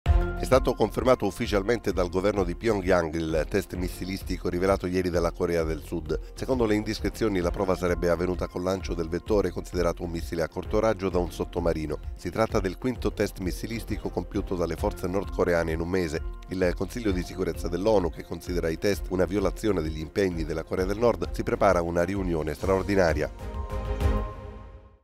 Pyongyang conferma il test missilistico rivelato da Seul
La prova effettuata col lancio da un sottomarino. Il Consiglio di Sicurezza dell'ONU verso una riunione straordinaria